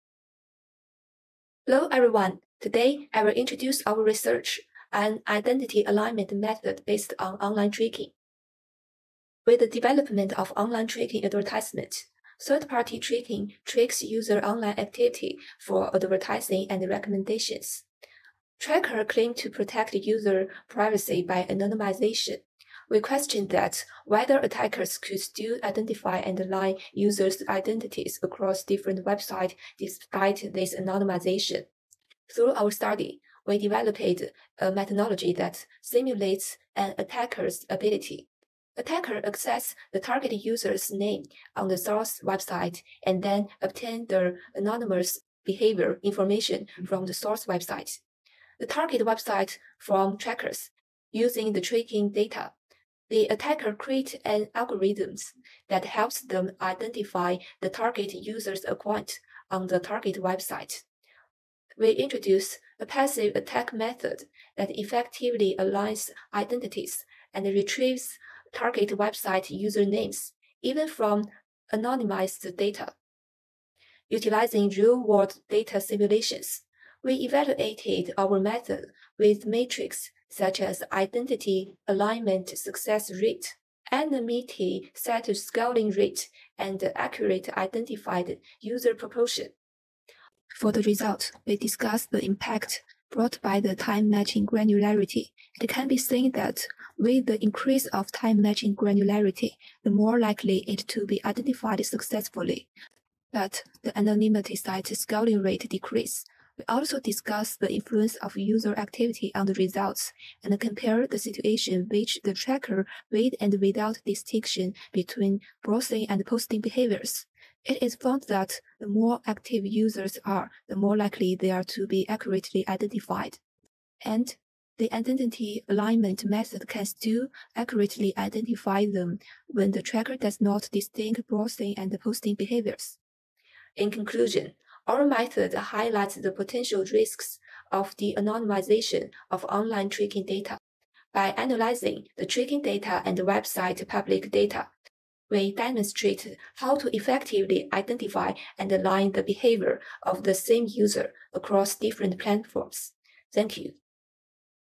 [shp2657] An Identity Alignment Method based on Online Tracking
"An Identity Alignment Method based on Online Tracking

Ruisheng Shi, Zhiyuan Peng, Tong Fu, Lina Lan, Jiaqi Zeng, Yuyang Shi, Jinqiao Shi, Shenwen Lin and Lin Li"